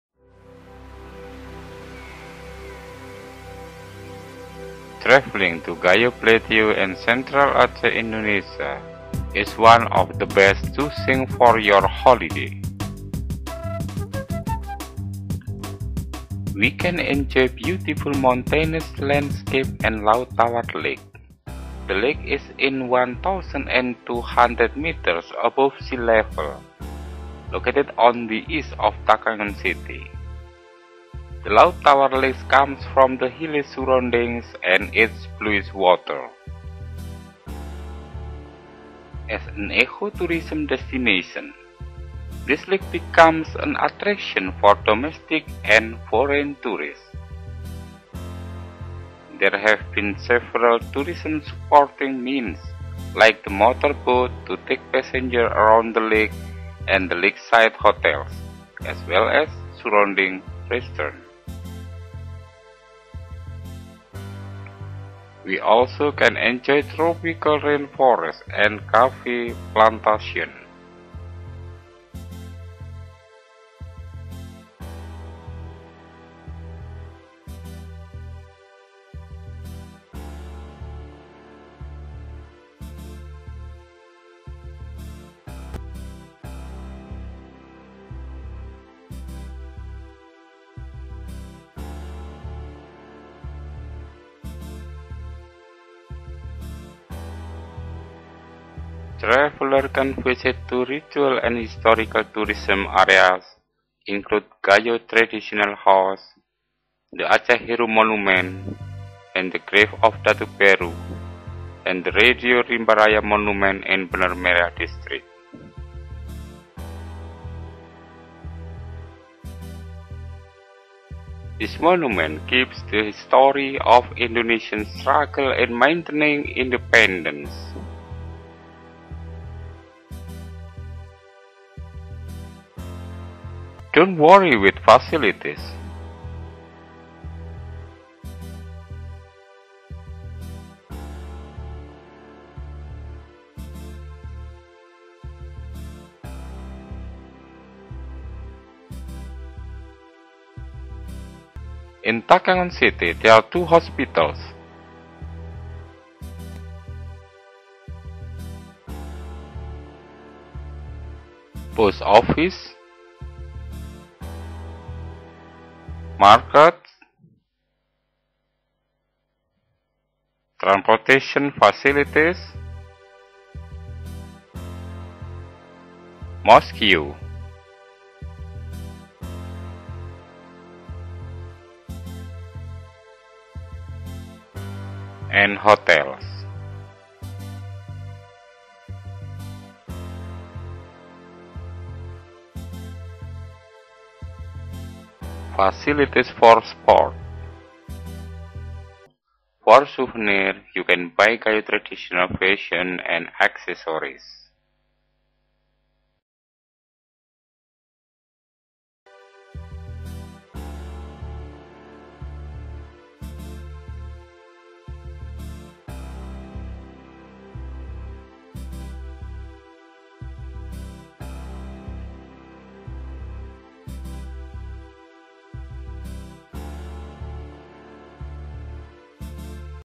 Traveling to Gayo Aceh
Traveling to Gayo plateau, in Central Aceh, Indonesia, is one of the best choosing for your holiday. Gayo Land is the main tourist destination in this country. We can enjoy beautiful mountainous landscape and Laut Tawar Lake. The lake is in one thousand and two hundred meters above sea level, located on the east of the Takengon City. The Laut Tawar Lake comes from the hilly surroundings and its bluish water. As an eco tourism destination, this lake becomes an attraction for domestic and foreign tourists. There have been several tourism supporting means like the motor boats to take passengers around the lake and the lakeside hotels, as well as surrounding restaurants. We also can enjoy tropical rainforests and coffee plantation. Dont worry with facilities. In Takengon City, there are two hospitals. This is Hospital Datu Beru which is located in Kebayakan street. Other facilities include bank, markets, and mosque. For stay, there are 12 hotels in Takengon.